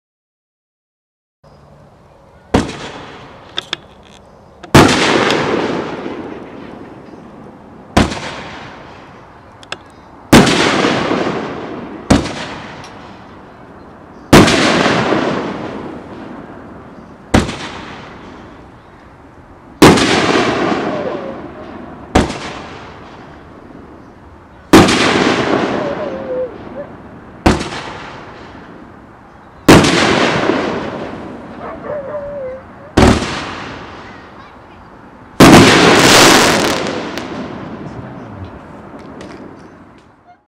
Pyro Box - Presidential Power
500g, 9 shot, by Pyro Box Fireworks.
Filmed at Wild Wilma's Fireworks May 2022 demo in Chuckey TN.